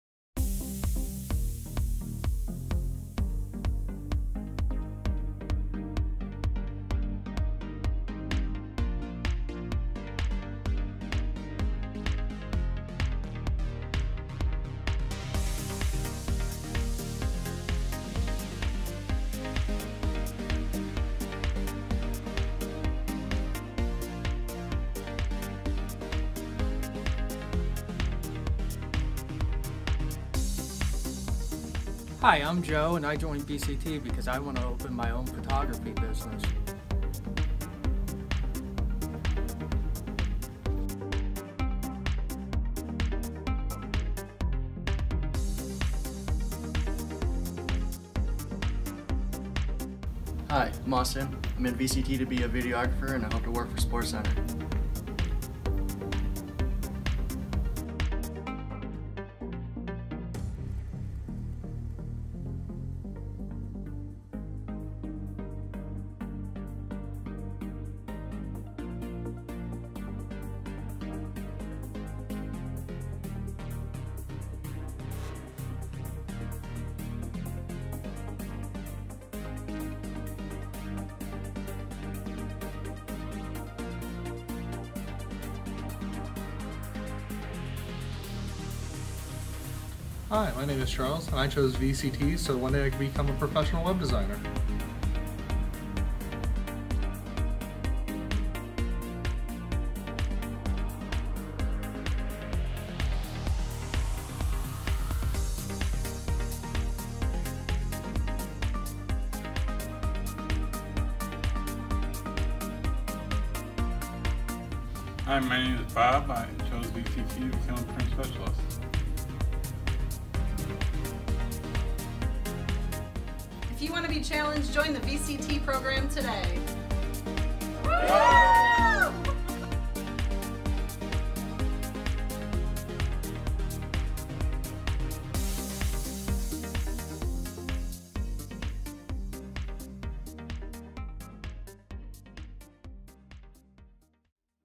Welcome to VCT at BGSU Firelands
The Intro to Video Production (VCT 2680) created a mannequin challenge video (with a twist) in order to show what we do in VCT and also to introduce a few of our students. The point was to show that students learn video, photography, interactive media, and print and we have fun doing so! The VCT 2680 students came up with the creative idea, scripted and storyboarded the idea, coordinated the talent, shot and edited the video.